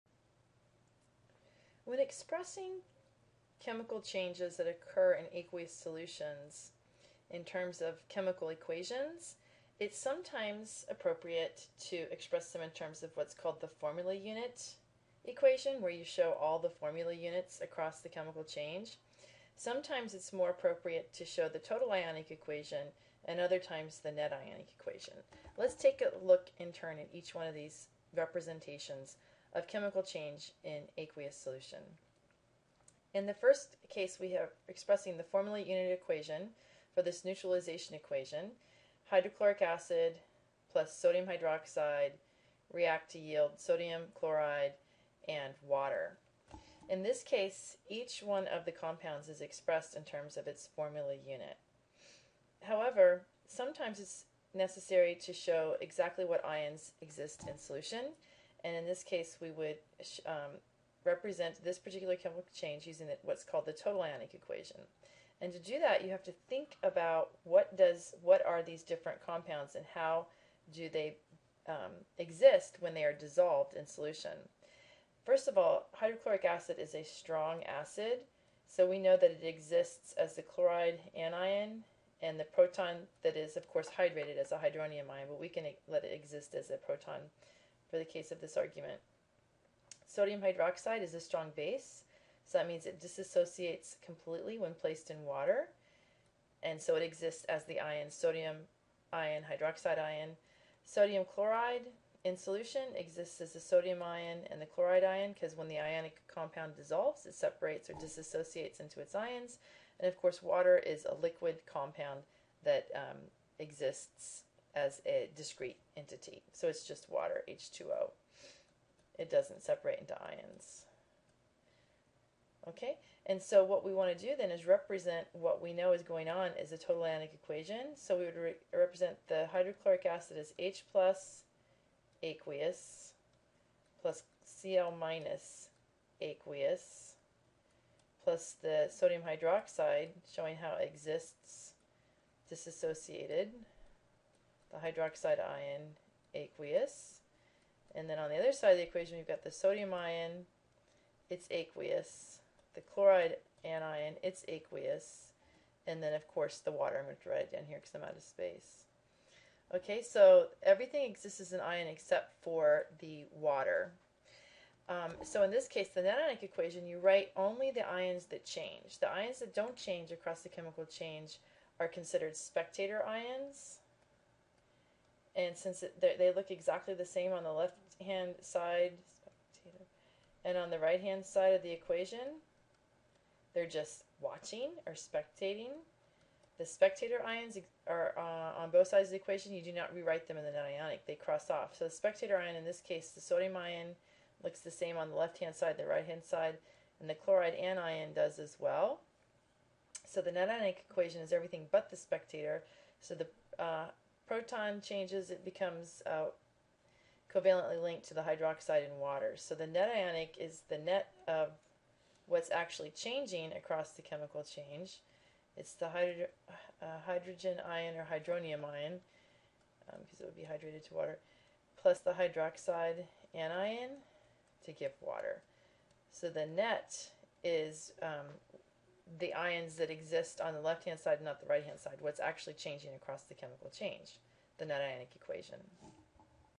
Ionic Equations, Net ionic Equations Defined
Definition of formula unit, total ionic and net ionic equations.